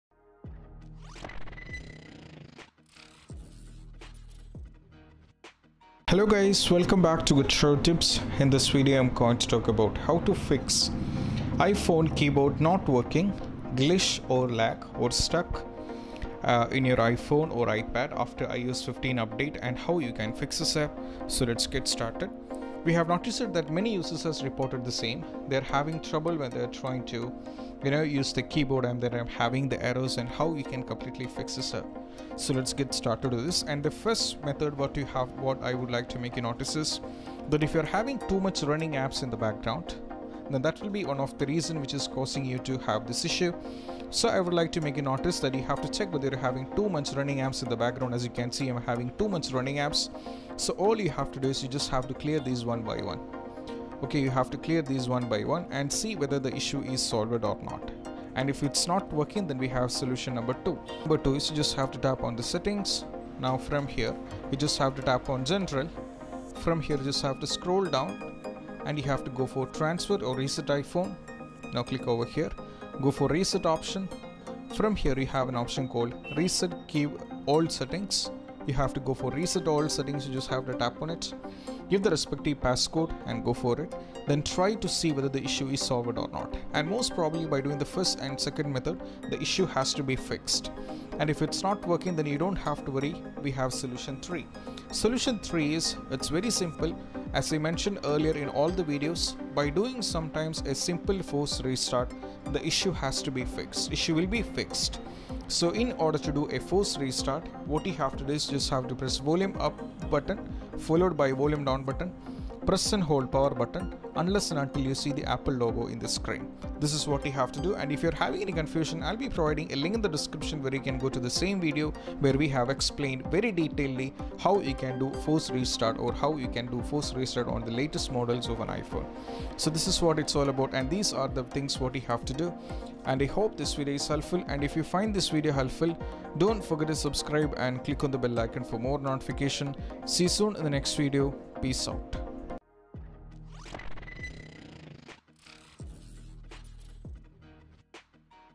How to Fix iPhone Keyboard Not Working/Glitch/Lag/Stuck iOS 15
Many users reported that they are facing lag and typing delays on keyboard in iOS 15/14. This issue is not only appeared in one app but also reported in a lot of apps including Safari browser, Facebook, Message etc. If you have the similar case, then your iMessage lagging or will be solved today. Here we have 4 solutions for this keyboard lag issue of iOS 15/14.

This Tutorial will guide you on How to Fix iPhone Keyboard Not Working/Glitch/Lag/Stuck iOS 15.